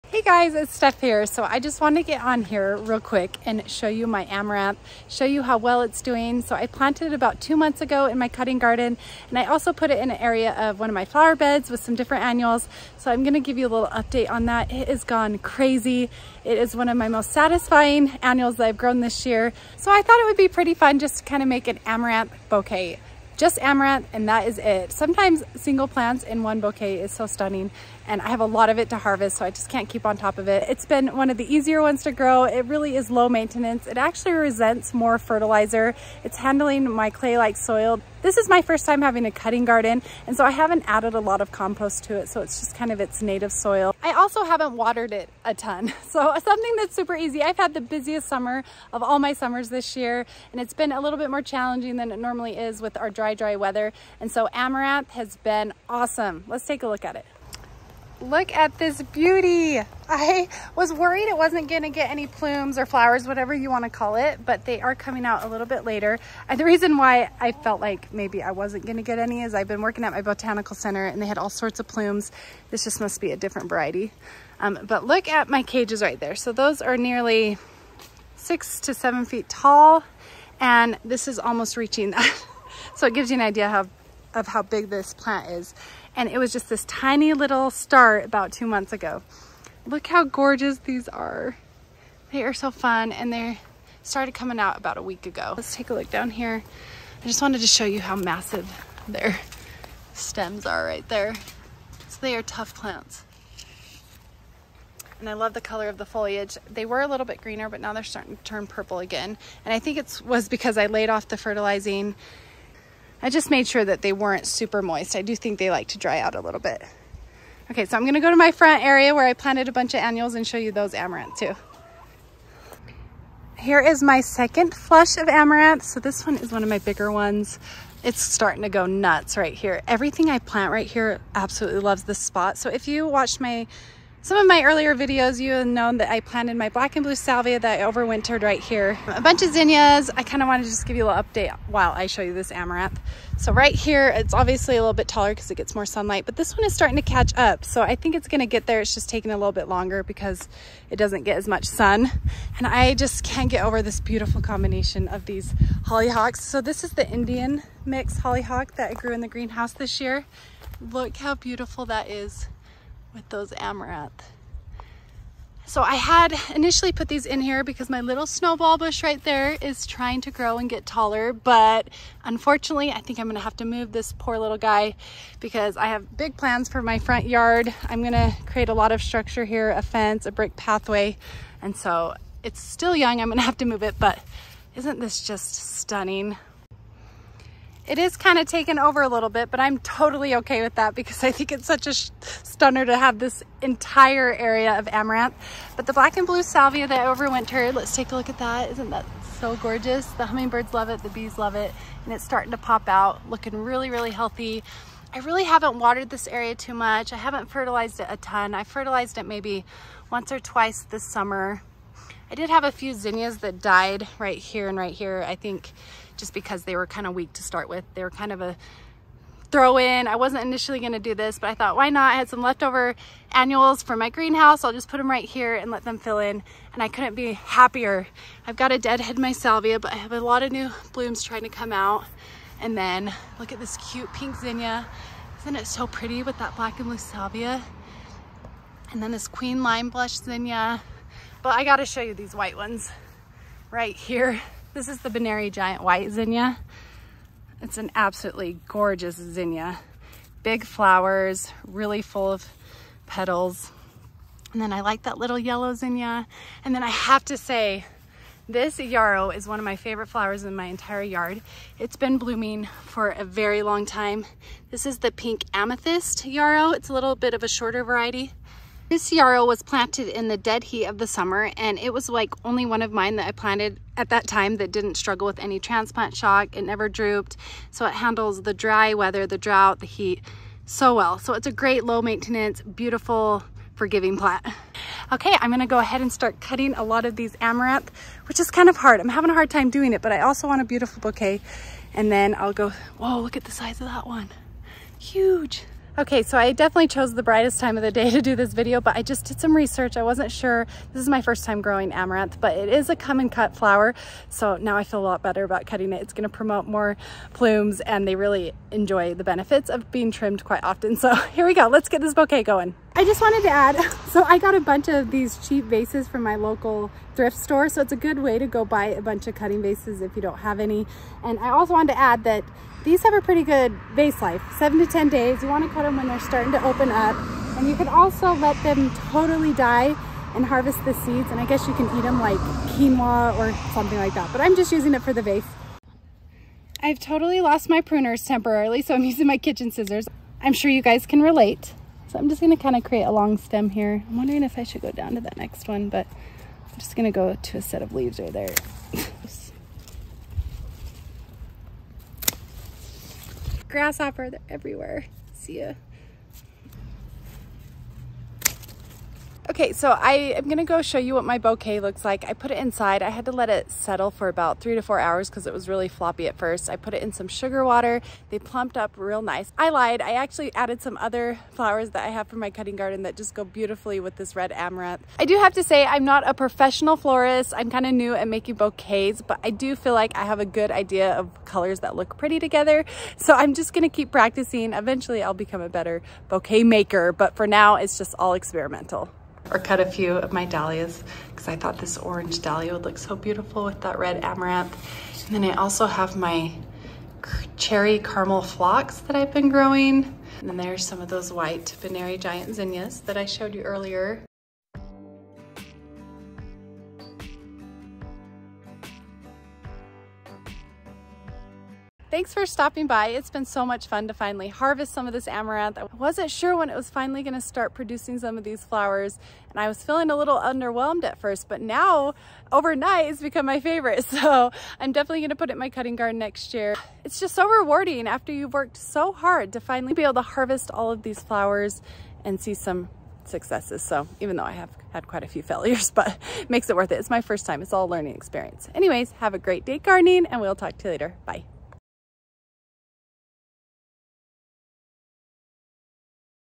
Red Spike Amaranth Highlight Video! Harvesting Amaranth and Annuals for a Bouquet. //FlowerFanatic
I have had great success with the Red Spike Amaranth this year. Since this was my first year growing it, I wasn't sure what to expect. The plumes took their  time to emerge, but now that they are out...they are going crazy. There are so many plumes to cut! I would definitely grow this Variety of Amaranth in your own garden if you want something that doesn't require a lot of love, takes up ALOT of space, and add fullness to your Vaces. It pairs really well with my Cherry Caramel Phlox, and Orange Dahlias. They have a long vace life (7-10 Days) and the dark color is hard to beat. I'm so glad I found an annual that likes my hot, dry, imperfect soil in my very first cutting garden! It makes me feel like a professional and I know it will make you feel that way too haha.
Steph AKA FlowerFanatic